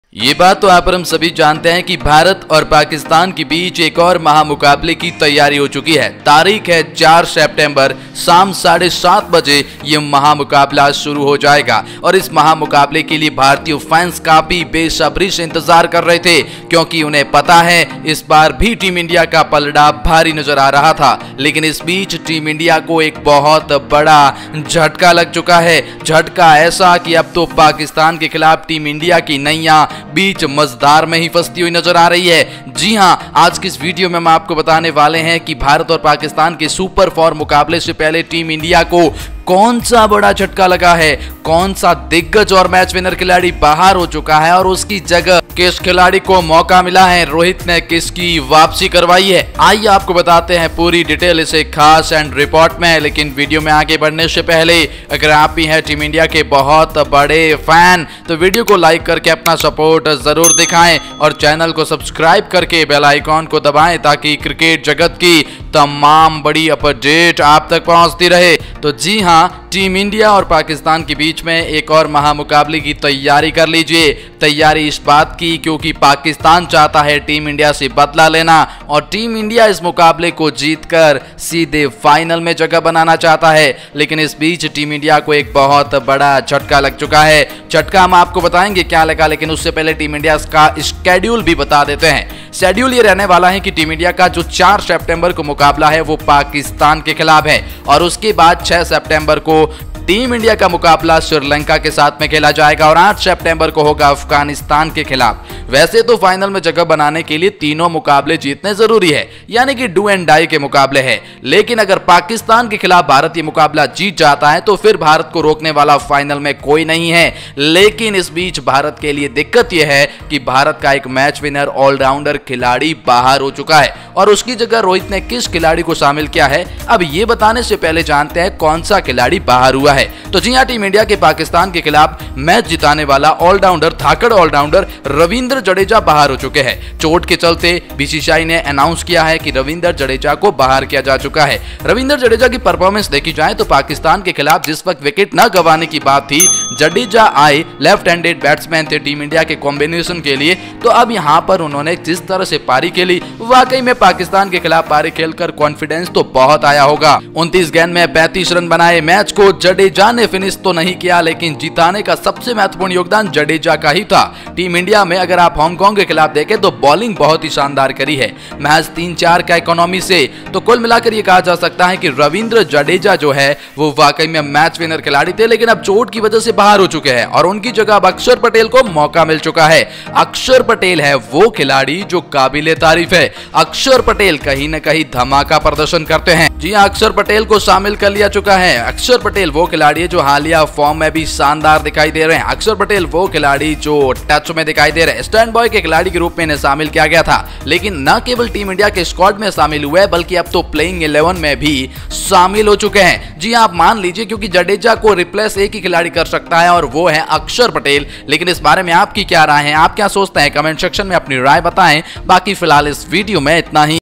Asia Cup 2022 Super 4 से पहले बढ़ी Team India की मुश्किलें, उसकी जगह हुई धाकड़ खिलाड़ी की एंट्री
Asia Cup 2022 Super 4 से पहले बढ़ी Team India की मुश्किलें, उसकी जगह हुई धाकड़ खिलाड़ी की एंट्री | India Vs Pakistan 2022 | India Playing 11 Against Pakistan 2022